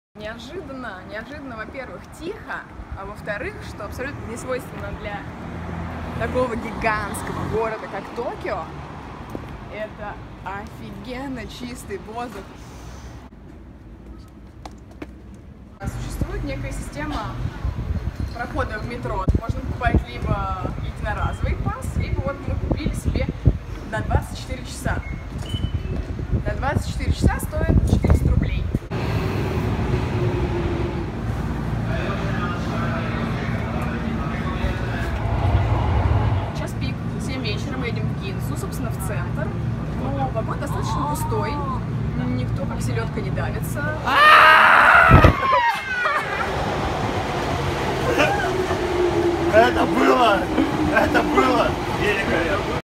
Мифы о метро в Токио!
Включайте качество 1080 HD! Токийское метро. Первый миф о Токио развенчан: адских давок в час пик нет. Информация о системе передвижения в токийском метро.